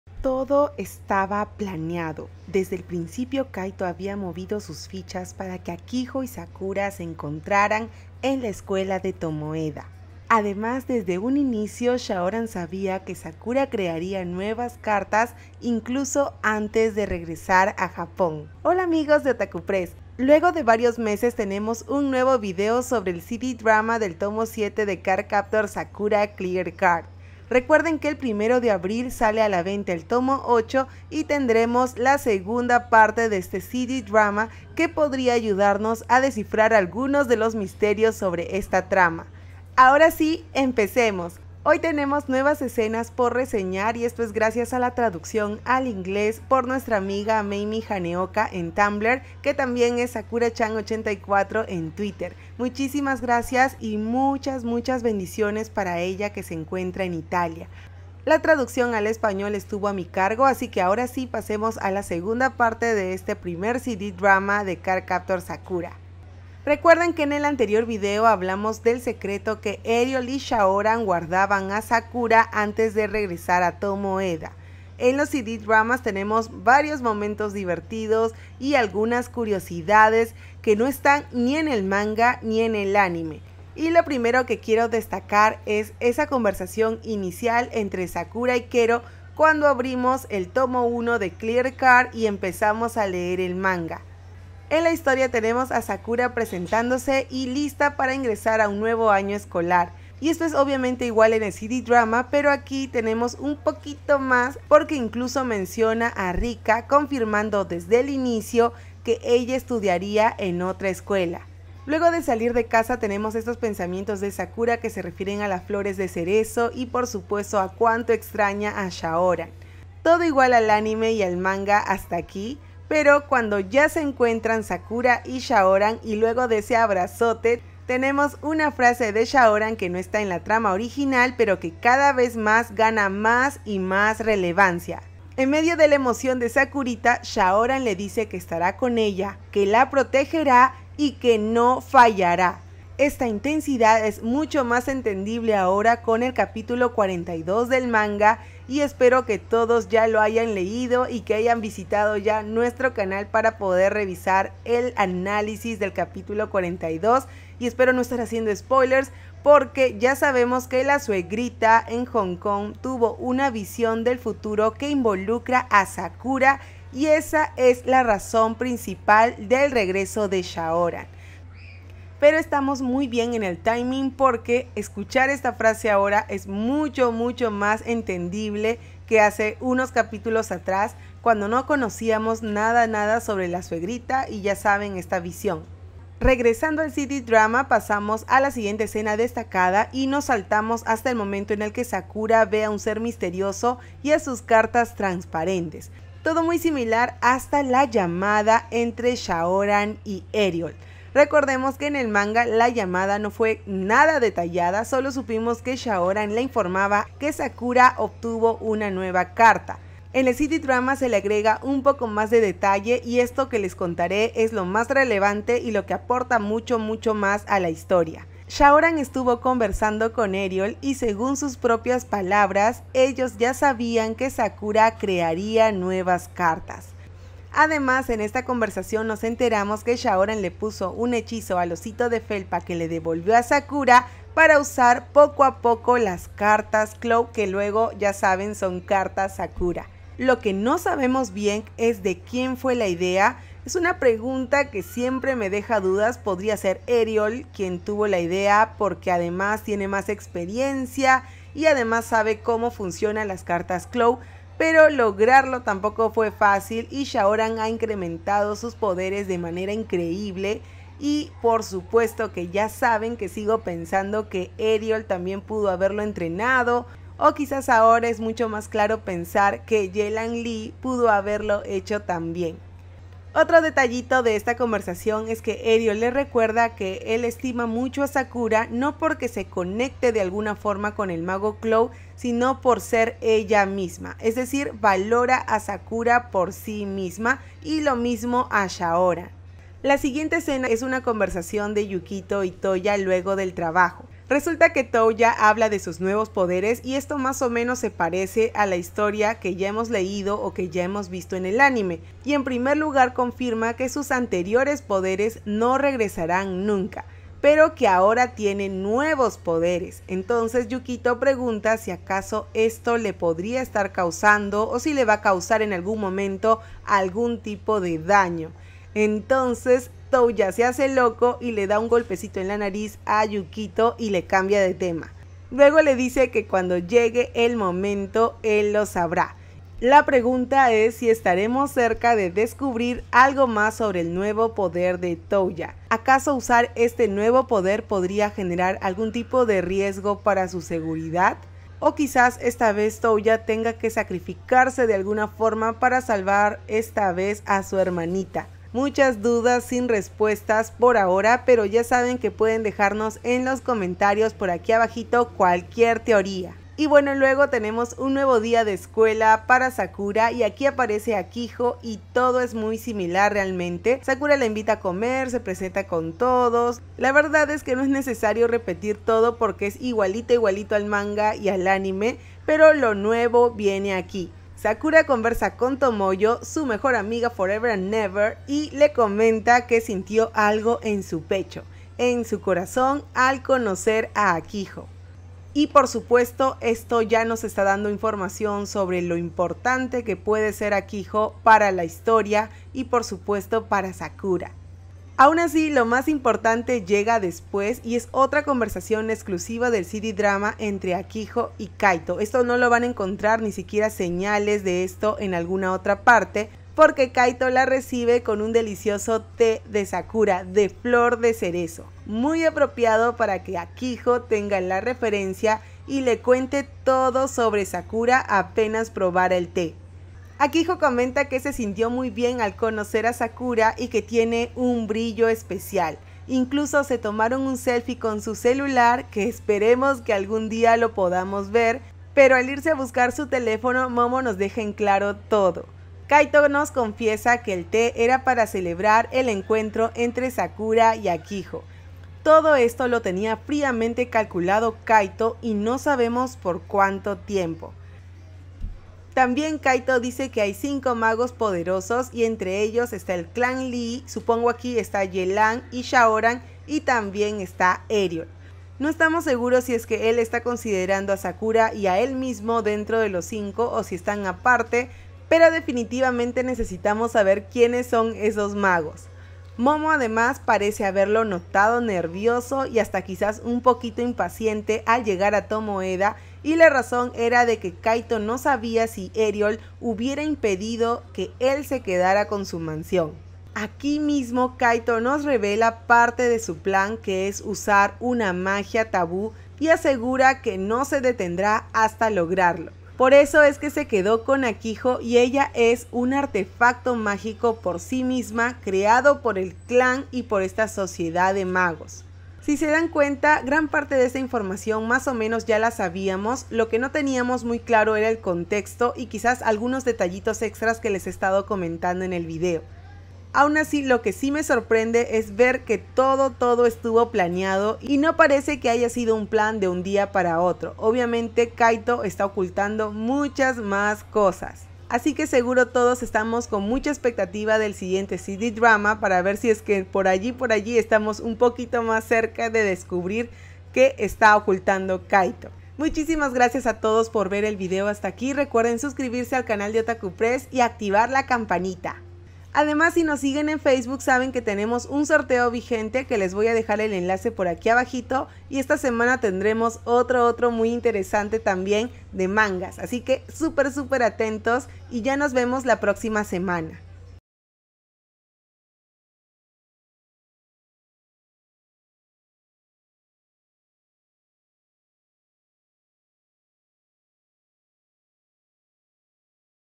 ENCUENTRO DE SAKURA Y AKIHO FUE PLANEADO POR KAITO | SAKURA CLEAR CARD | CD DRAMA PARTE 2
El CD Drama del tomo 7 de Cardcaptor Sakura: Clear Card tiene algunos momentos exclusivos que no aparecen ni en el manga ni en el anime.

En este video les cuento las curiosidades más destacadas. Recuerden que si tienen teorías, pueden dejarlas en los comentarios para el próximo Directo!

La segunda parte del CD Drama sale con el tomo 8 en Japón.

Curiosidades de este video:

- Gracias a @SakuraChan84 en Twitter. Visiten su blog en Tumblr con la traducción completa al inglés del CD Drama del tomo 7

¿Buscas productos oficiales de anime? Revisa la tienda de Otaku Press en Facebook (envío internacional)